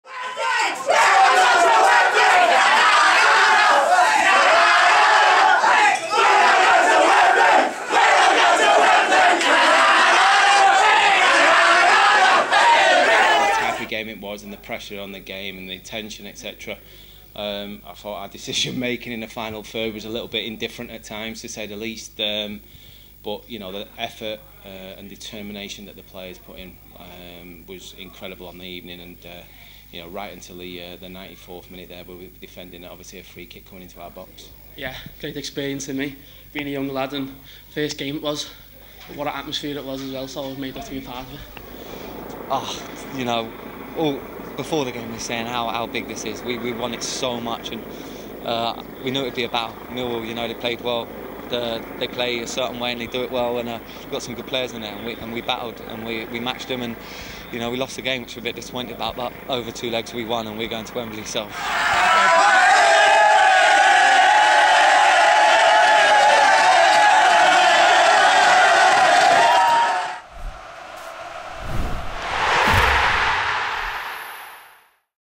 Oxford United Reach Wembley
Reaction as the U's beat Millwall to reach the Johnstone's Paint Trophy FInal